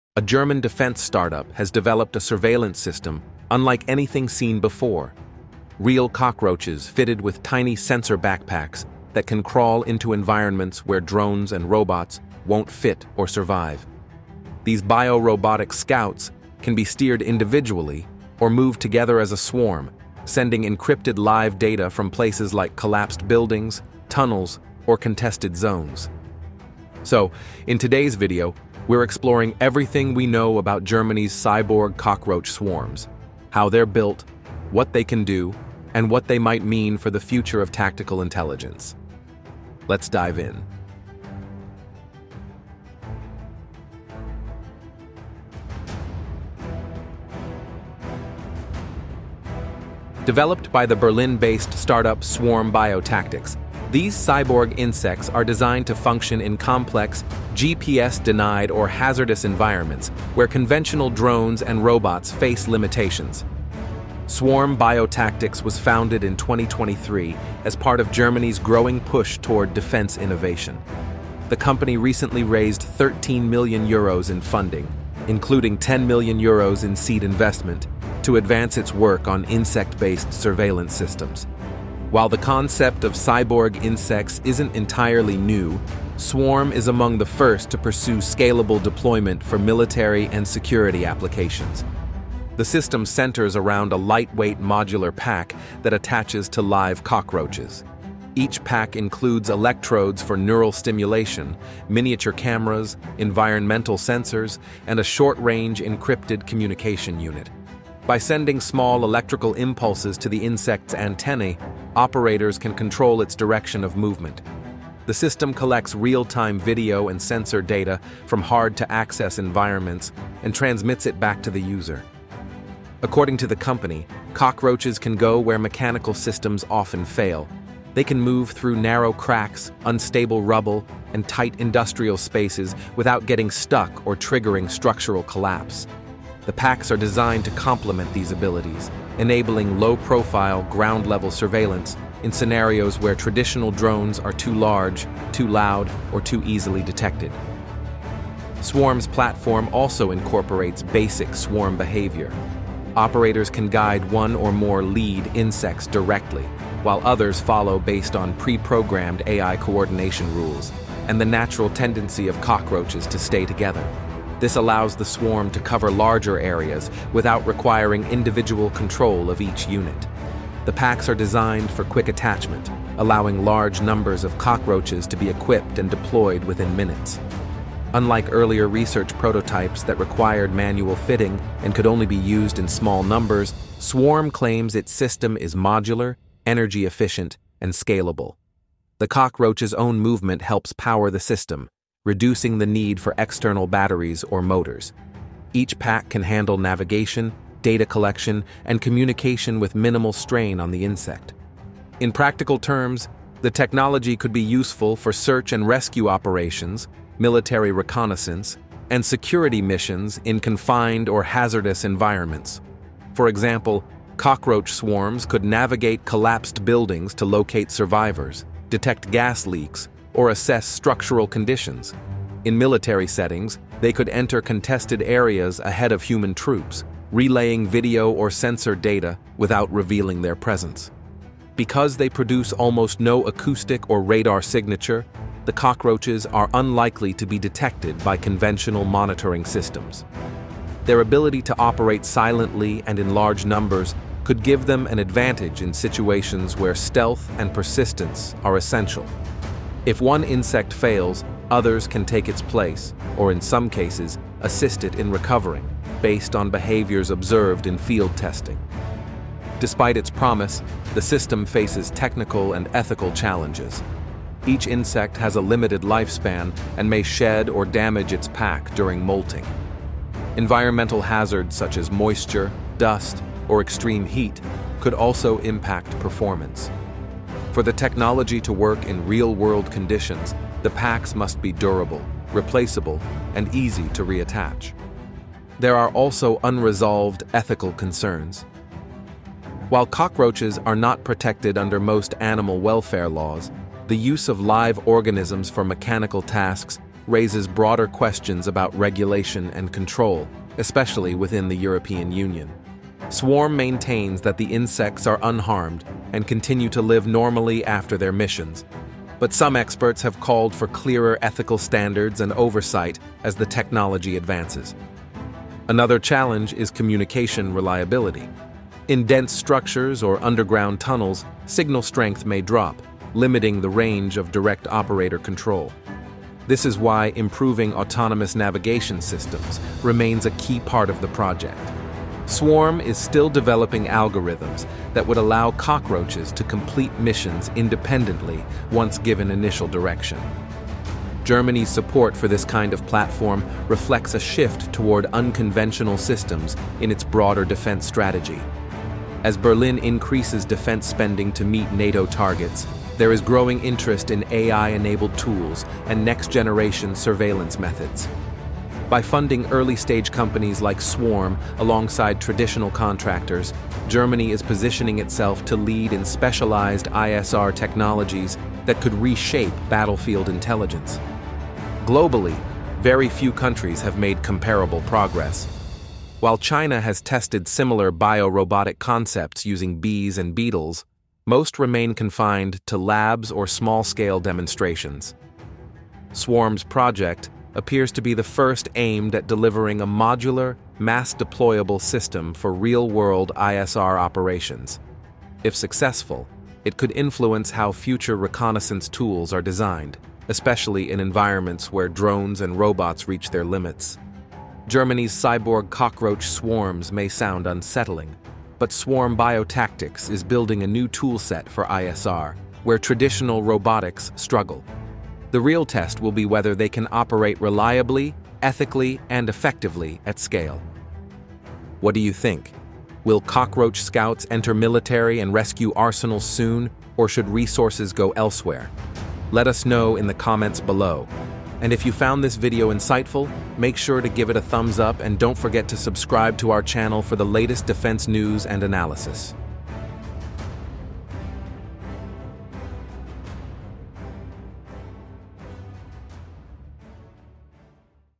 AI-Powered Cockroach Drones Are Here
Germany has unveiled a new defense project that turns real cockroaches into controllable drones. Fitted with tiny sensor packs and guided through electrical impulses, these insects can move through tunnels, rubble, and tight spaces where traditional drones cannot operate. Their ability to work alone or in swarms offers a new way to gather intelligence in contested environments. In this video, we take a closer look at Germany’s cyborg cockroach swarms, how they are being developed, and what they could mean for the future of military surveillance.

Defence Central does not own the rights to these pictures.